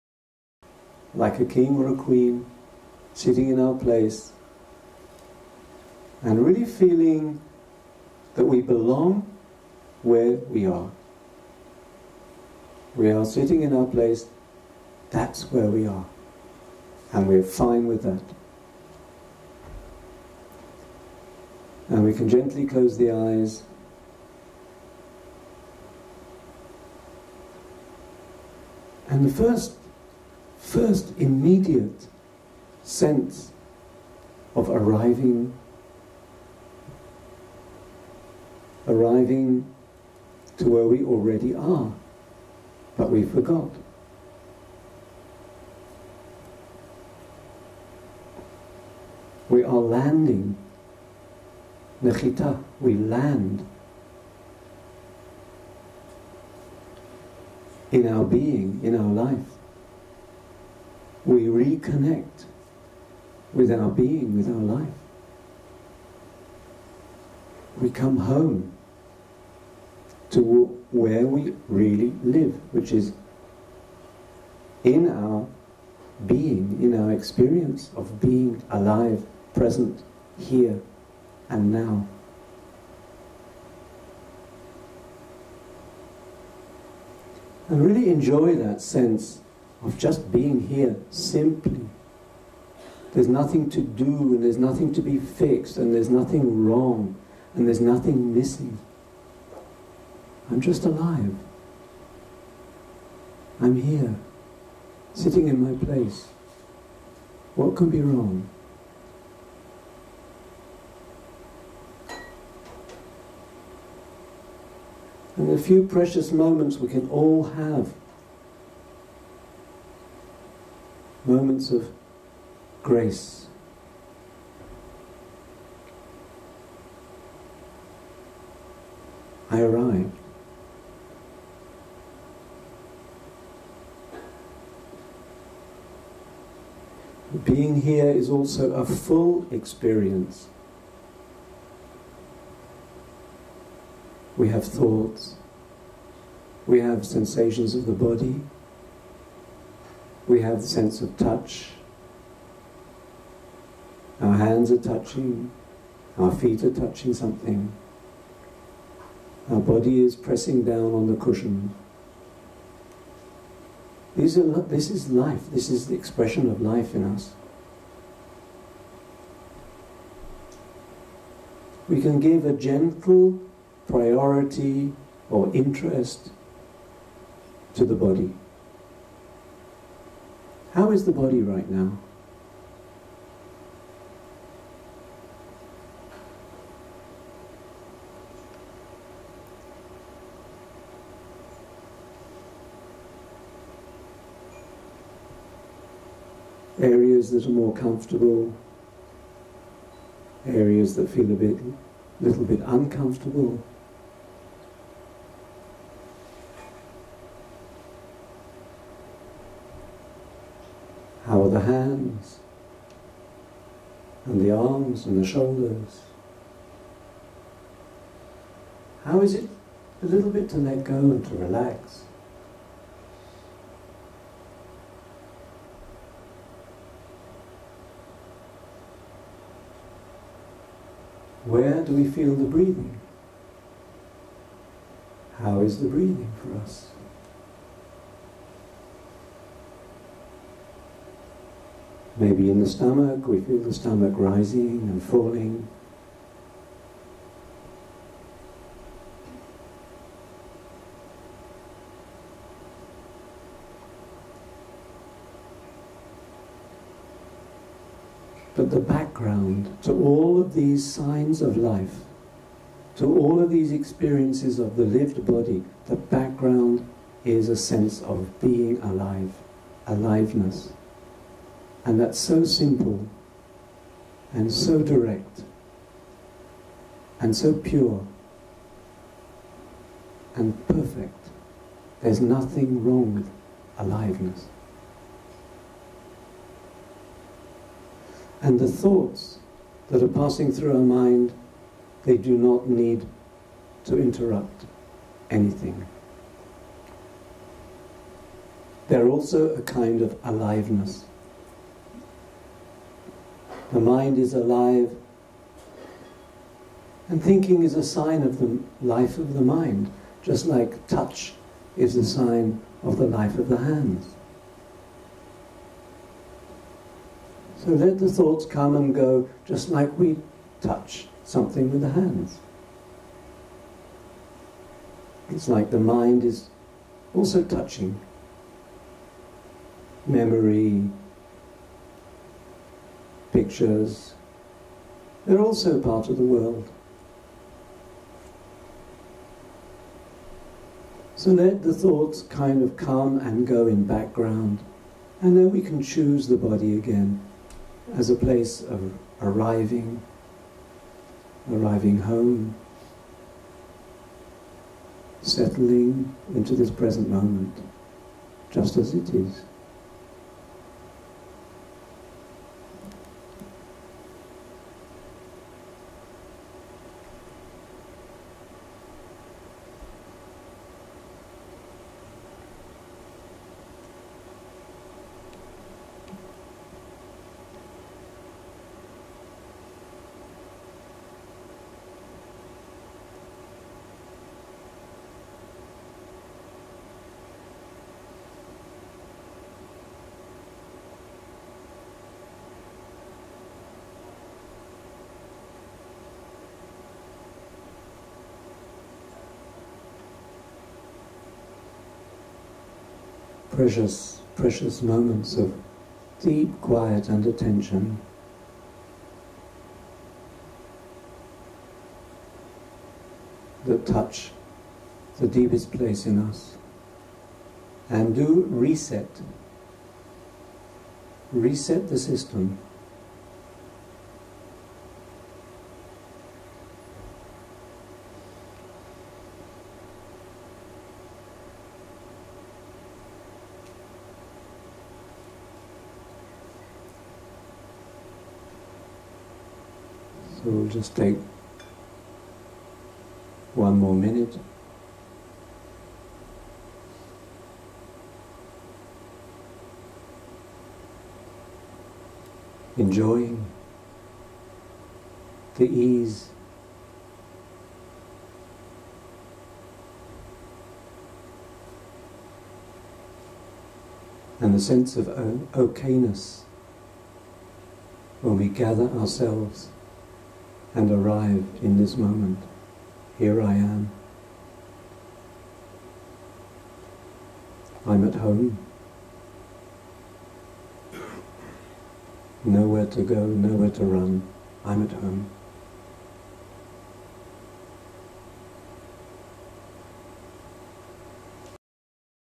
Guided Meditation on Joy, Well Being and Presence.
This is a short meditation focusing on arriving into the present moment, and exploring it as a field of awareness. We can discover what it is like to come home to our life. We can discover the subtle dimension of bodily life and the taste of joy that is waiting for us when we connect intimately with our lived body. We do not need previous experience, but we can see this as a hint of what meditation can be like for us and where it can take us.